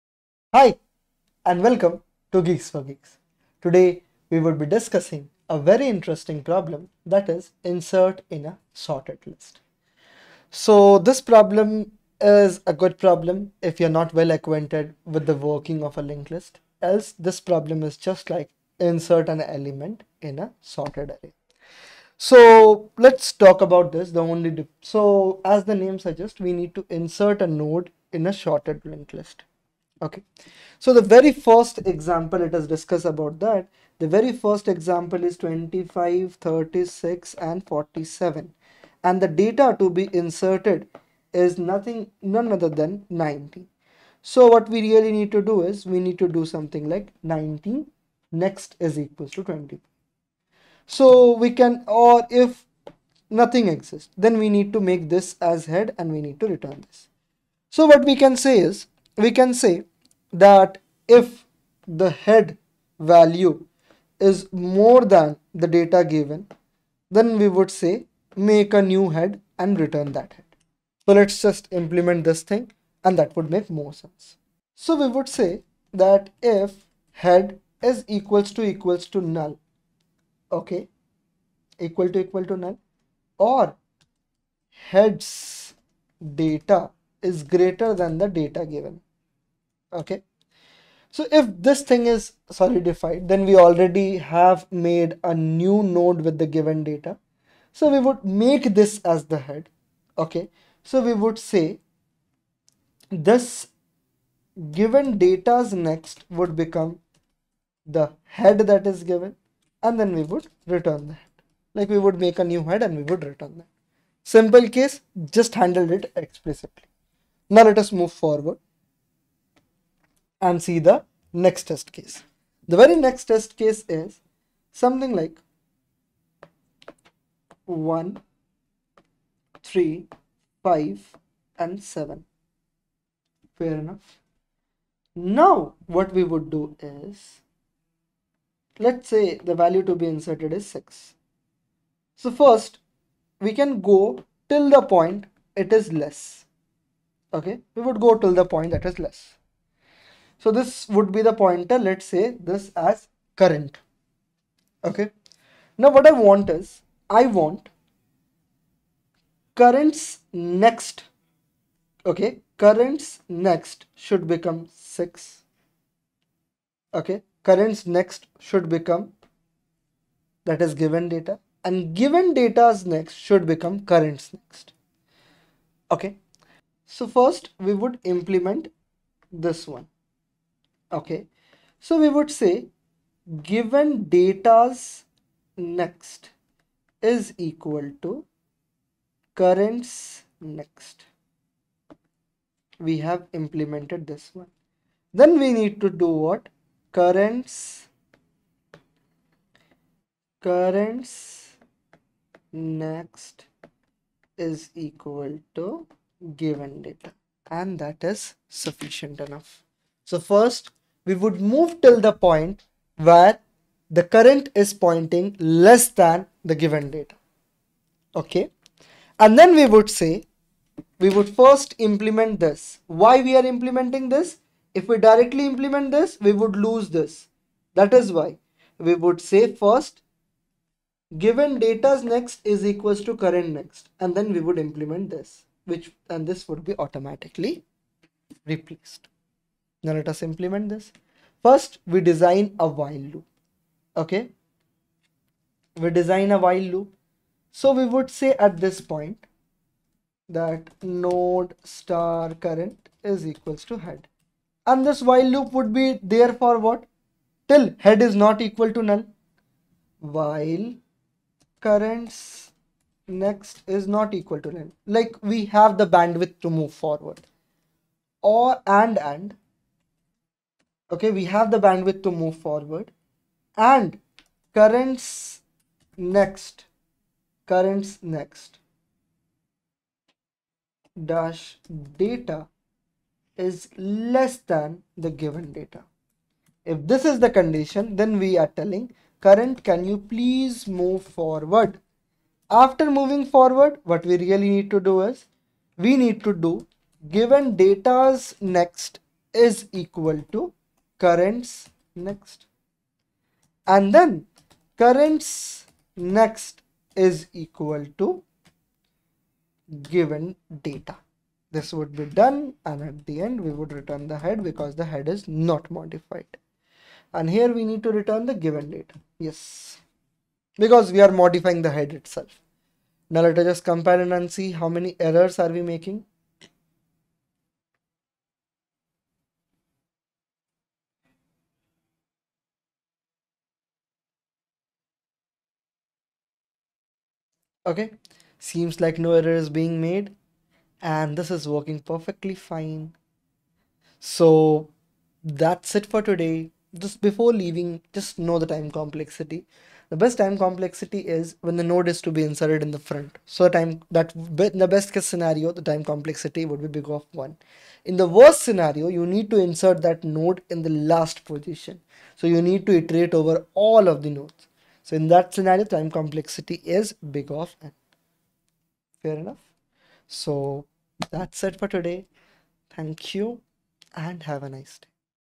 GFG POTD: 08/10/2023 | Insert in a Sorted List | Problem of the Day GeeksforGeeks
Welcome to the daily solving of our PROBLEM OF THE DAY with Siddharth Hazra. We will discuss the entire problem step-by-step and work towards developing an optimized solution. This will not only help you brush up on your concepts of Data Structures and Algorithms but will also help you build up problem-solving skills.

So come along and solve the GFG POTD of 08th October 2023 with us!

Find daily solutions for POTD here on our channel!